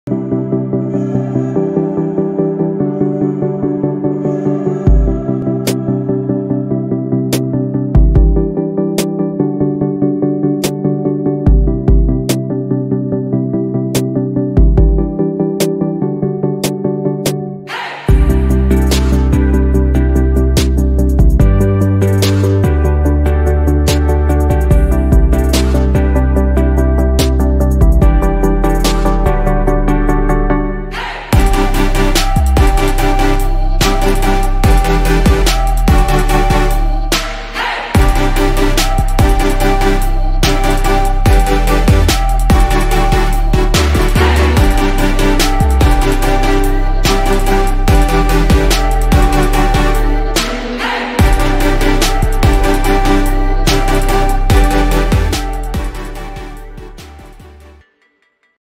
💬 Témoignage Installux sur la solution showroom virtuel
Retail VR a permis à la marque Installux de résoudre sa problématique grâce à la solution showroom virtuel !

Quelle était cette problématique de départ et comment Retail VR a permis d'y répondre ?